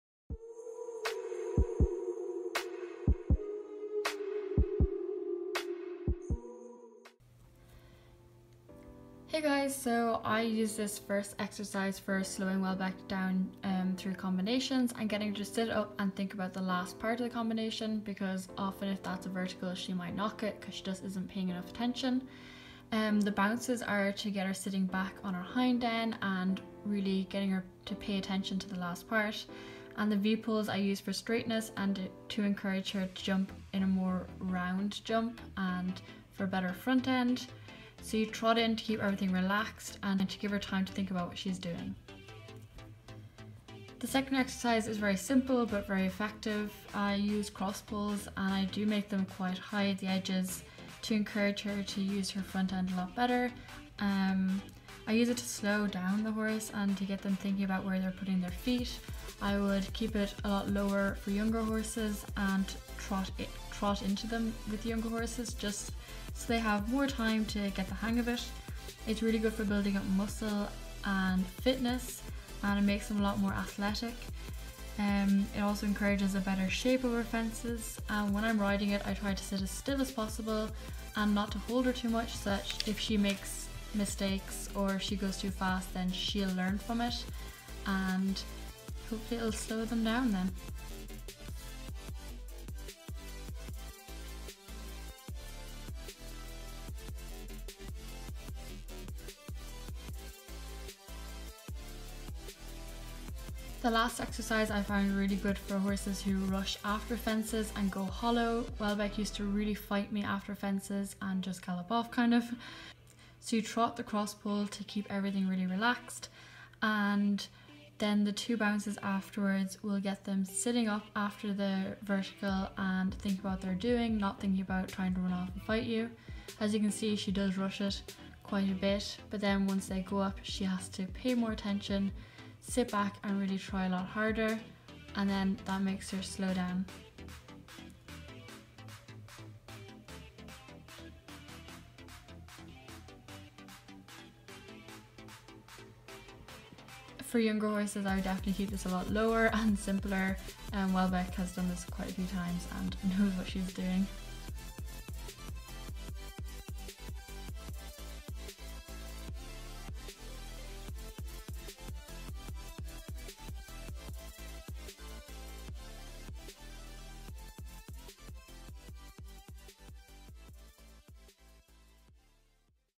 - 3 Showjumping Exercises -
›› Distances:
I measure the distances using my own steps so they'll be different for everyone, you're better off looking up the distance in meters and figuring out how many steps that is for you. As a rule of thumb, I have four steps = one stride, one step for take off and two for landing. But this depends on the height of the fences
Exercise 1: two bounces, one stride, verticle/oxer
Exercise 2: four bounces (three steps between each one, make the distance narrower to make it harder)
Exercise 3: cross pole, one stride, vertical, one stride, two bounces

›› So this was requested on my instagram and it took me a little while to get the clips together but I hope you guys enjoy it and you get some ideas maybe? I was thinking of filming showjumping exercises for young horses using Indie if ye would be interested in that? Leave any requests in the comments!

›› Camera Info:
Nikon d5500 w/ Sigma 17-70mm
iPhone 6s